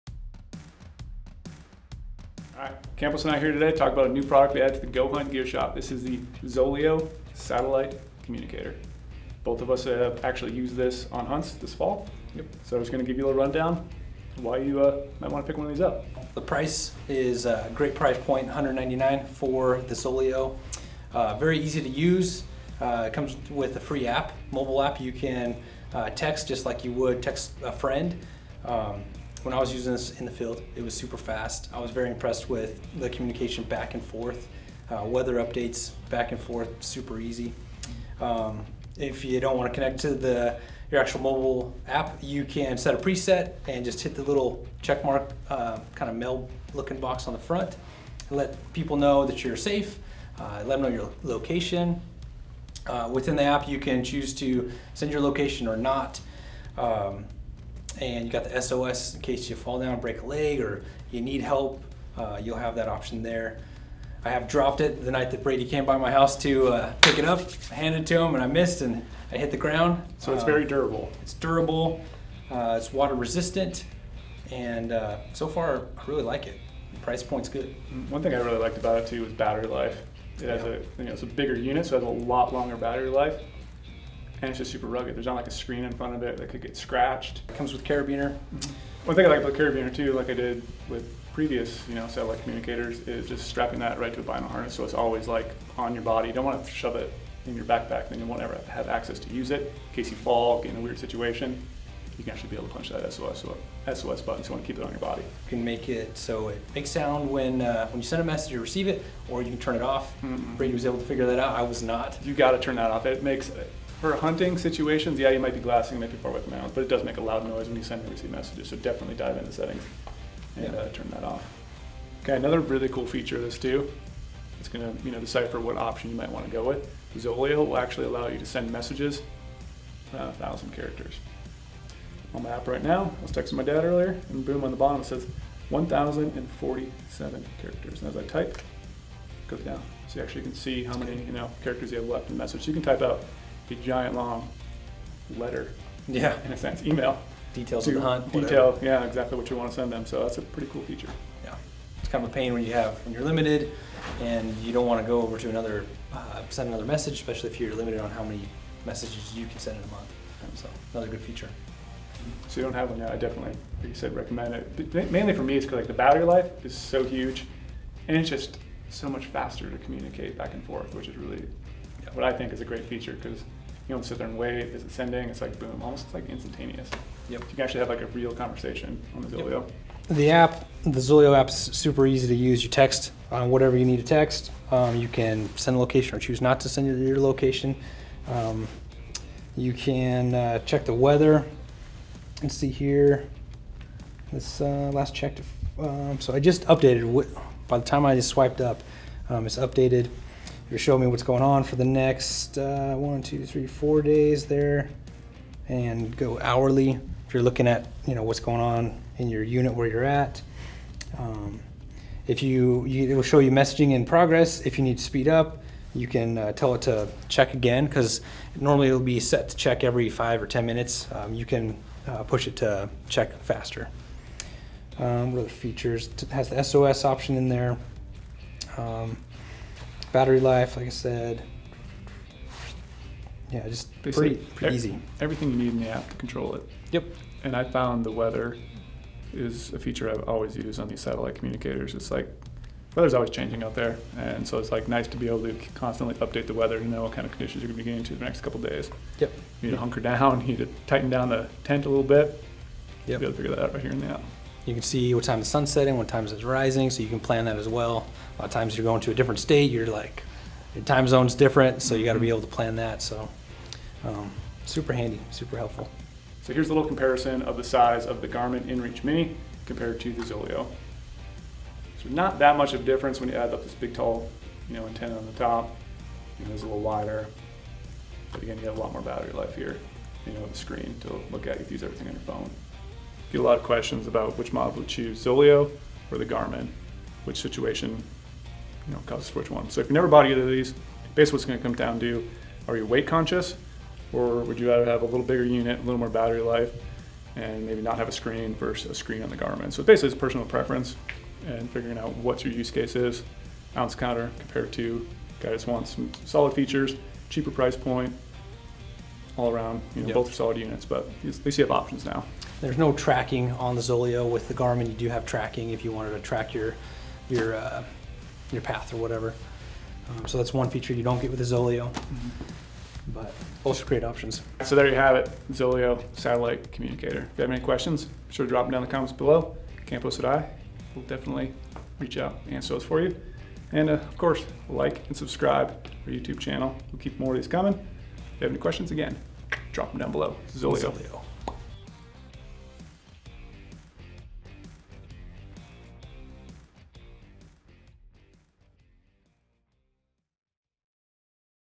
Stay Connected in the Backcountry - Zoleo Satellite Communicator
The Zoleo is an easy-to-use satellite communicator meant to keep you in touch while off-grid. The device pairs seamlessly with a phone app for quick messaging. You can now find the product in the goHUNT gear shop!